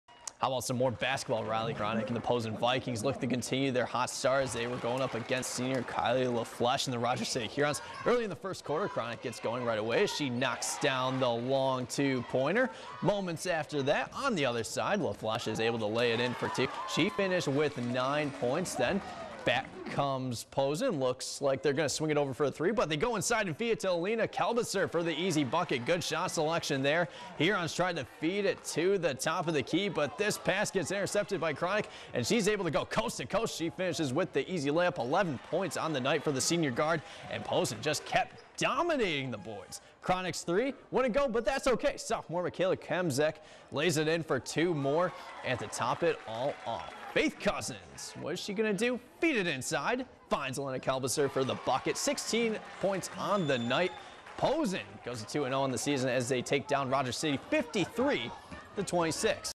Vikings top Hurons 53-26
The Posen Vikings were able to steal another game on the road as they crushed Rogers City 53-26.